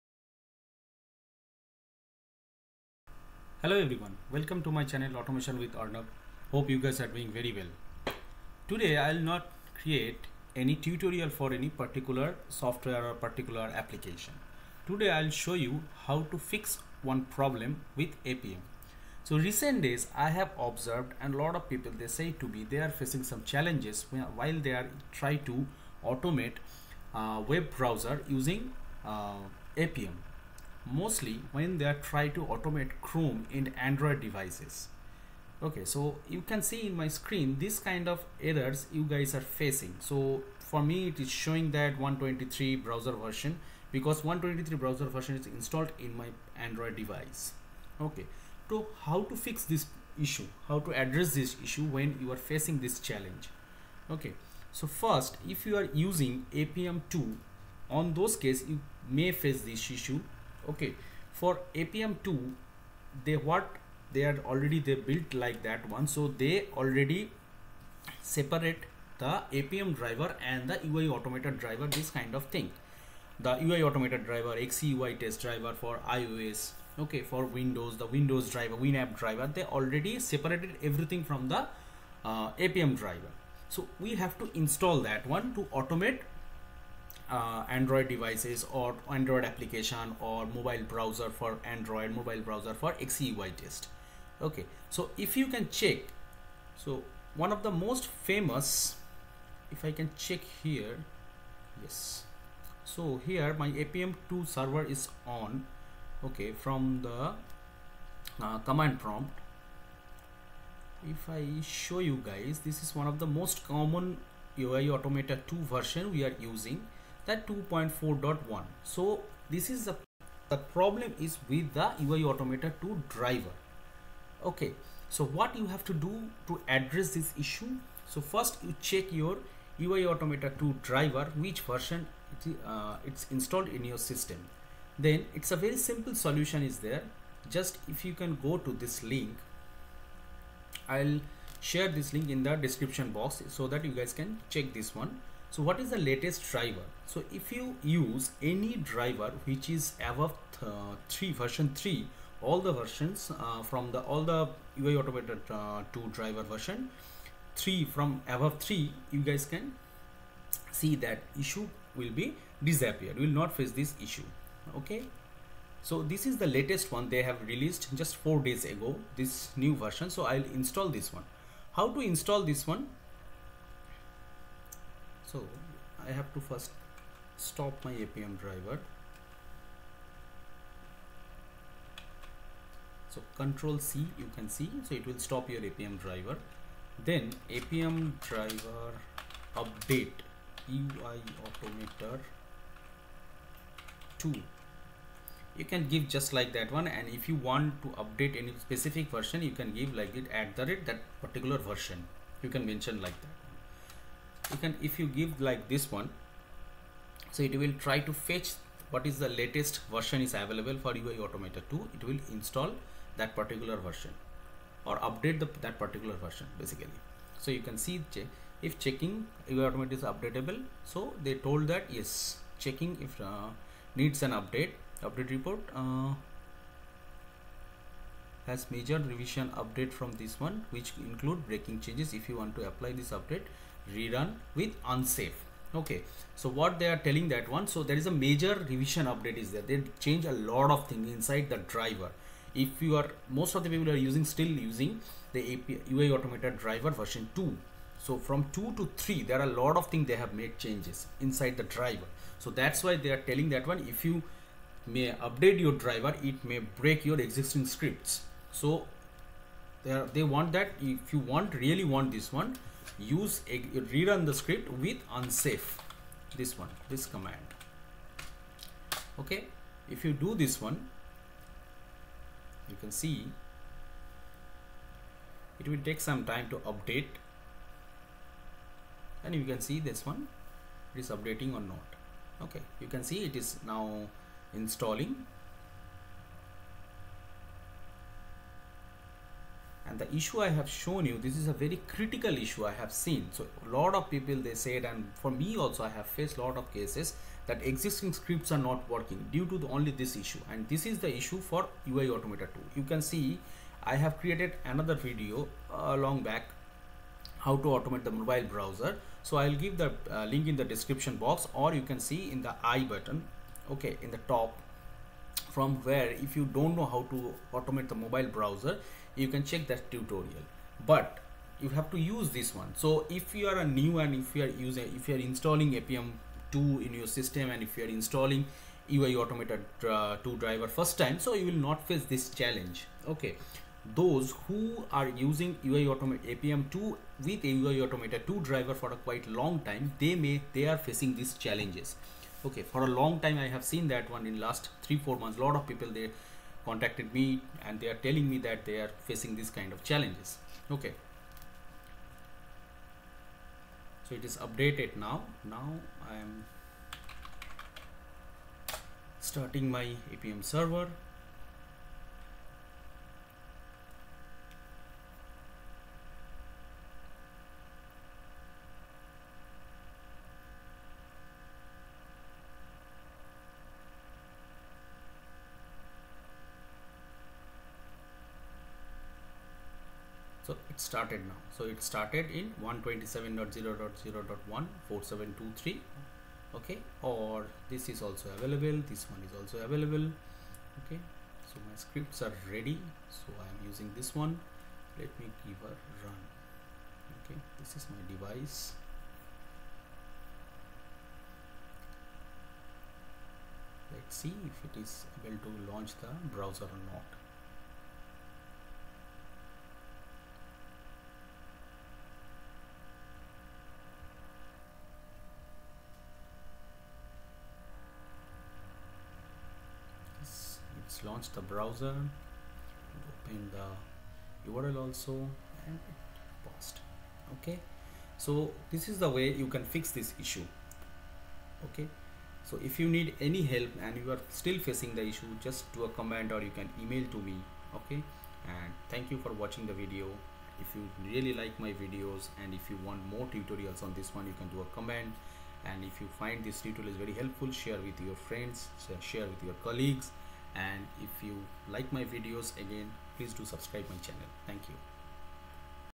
| No Chromedriver found that can automate Chrome Appium Issue | UIAutomator2 | Appium 2 |
The solution to "An unknown server-side error occurred while processing the command" was covered in this video. Initial error: Using appium 2 and uiAutomator2, no Chromedriver could be discovered to automate Chrome '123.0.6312'."